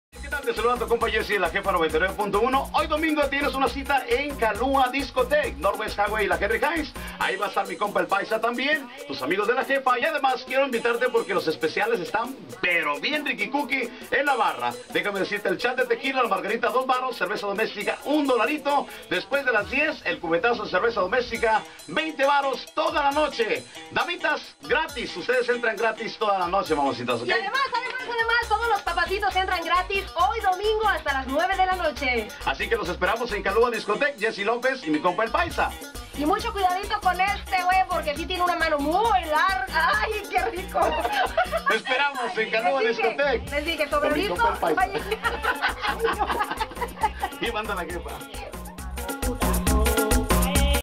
Los Domingos Son Diferentes Con La Jefa 99.1Fm Jesse Lopez Y El Paisa En KALUA
Esta Noche Los Esperamos En KALUA DISCO Mas Detalles Checate A JESSE LOPEZ EL MANOTAS ( ( En Este Video ) ) No Pudo Contenerse Con La RIKICOOKIES De TERE LA BABY ;)